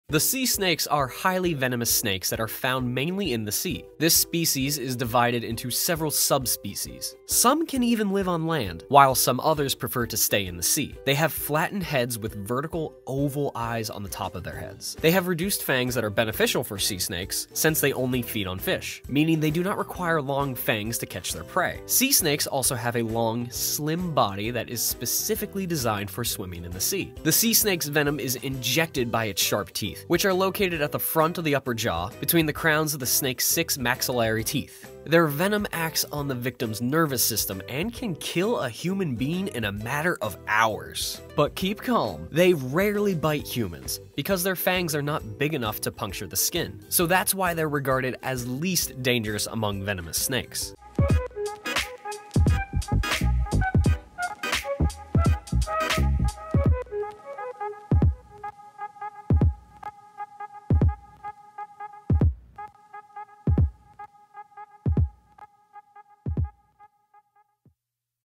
Sea Snake - In 1 Minute! 🐍 One Of The Most Dangerous Ocean Creatures In The World | 1 Minute Animals
Sea Snakes are venomous snakes found in the ocean and are considered some of the most dangerous creatures in the world. They have adapted to life in the water, with a flattened tail and paddle-like scales that help them swim. Sea Snakes are known to be highly venomous and their bites can cause paralysis, heart failure, and death. However, they are not aggressive and bites are rare, typically occurring when they feel threatened or accidentally disturbed. Sea Snakes feed on fish and eels, and are found in tropical and subtropical waters, particularly around the coasts of Southeast Asia and Australia.